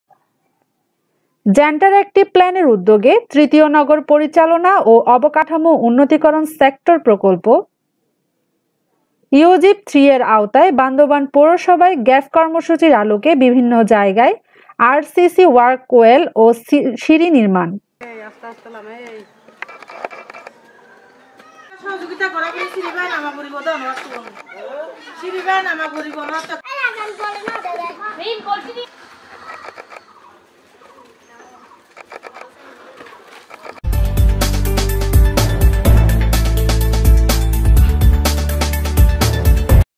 জেন্টার একটভি প্লানরে উদ্যোগে তৃতীয় নগর পরচিালন ও অবকাঠামো উন্নতকিরণ প্রকল্প -৩
জন্টোর একটভি প্লানরে উদ্যোগে তৃতীয় নগর পরচিালন ও অবকাঠামো উন্নতকিরণ (সেক্টর) প্রকল্প গফে... UGIIP-3 এর আওতায় বান্দরবান পৌরসভায় গেফ র্কমসূচীর আলোকে বিভিন্ন জায়গায় আর.সি.সি ওয়লে ও সিঁড়ি নর্মিাণ।